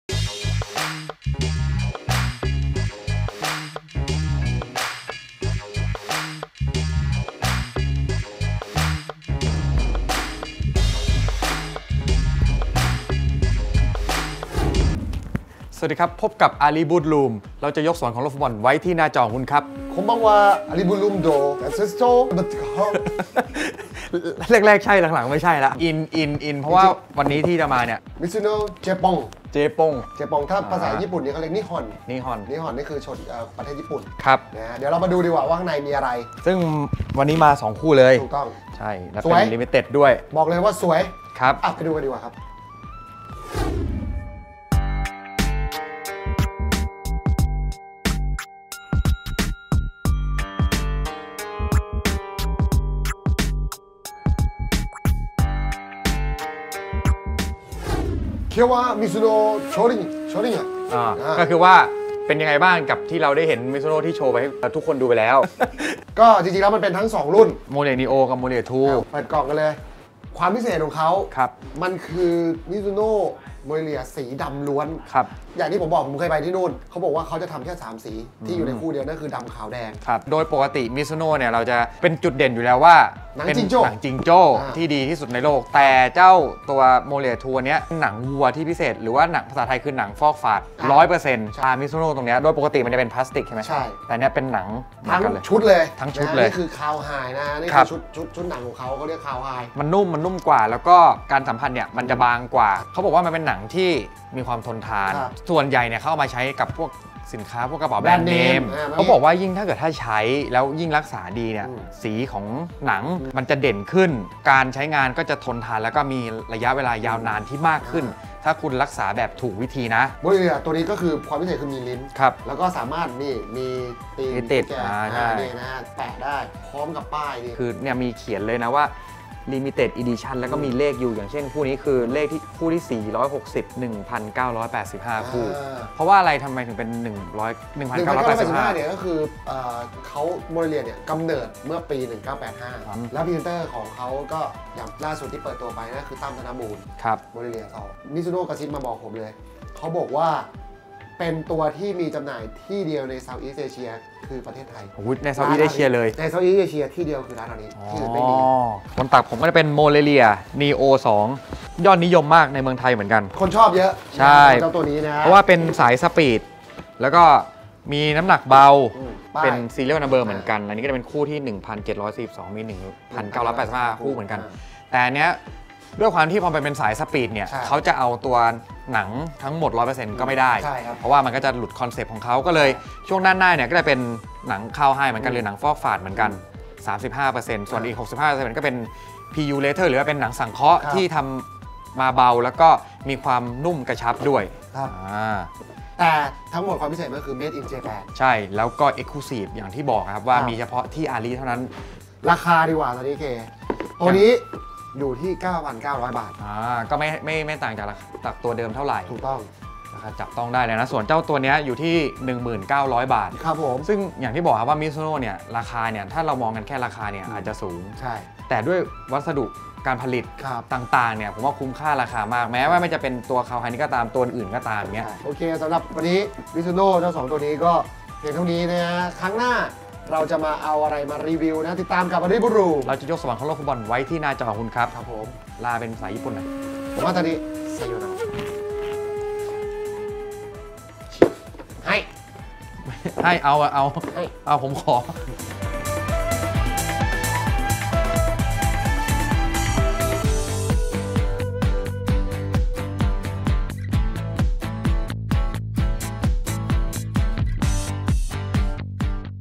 Mizuno "Japan Leather" By Ari Bootroom ⚽🇯🇵🎌
Ari_Exclusive : ที่เดียวในประเทศไทย 🇹🇭
พิเศษสุดๆ 1,985 คู่ทั่วโลก🔥🌎

ตามไปดูกันว่าเจ้ารองเท้า Mizuno Football "Japan Leather" ⚽🇯🇵🎌
สีใหม่ล่าสุด สวยเข้ม ดุดัน มีที่มาอย่างไร และทำไม⁉️
พบกันที่อาริฟุตบอล ทุกสาขา ในวันที่ 20 กรกฎาคม 2561 นี้
ช่องทางออนไลน์ arifootballstore.com
(ส่งฟรีทั่วประเทศ และมีบริการเก็บเงินปลายทาง) 🚛📦🏡